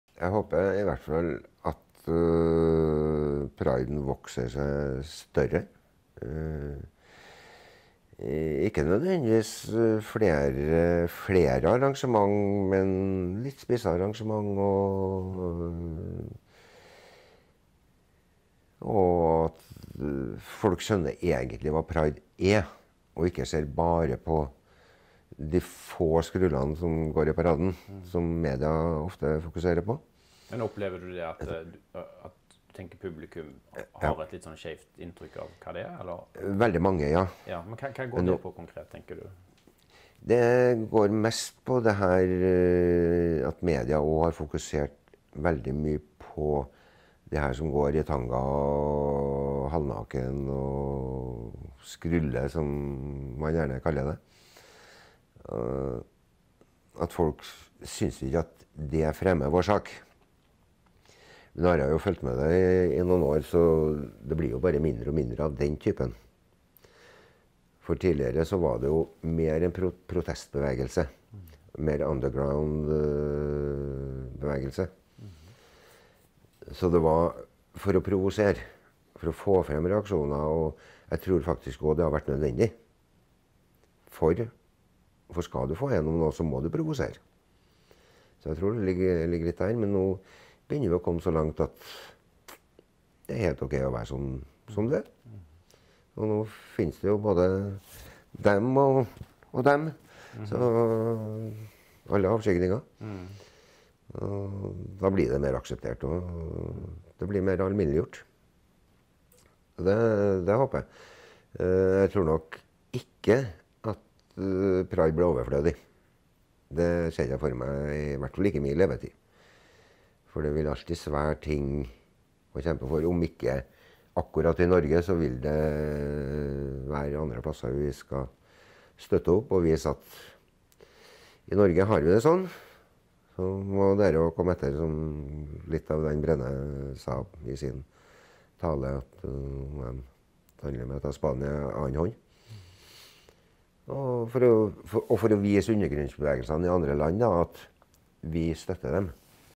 - Media fokuserer på de som «skruller».
Sverre Ohlgren tror ikke Pride blir overflødig. Han har vært "Pride-general" i Trondheim i flere år.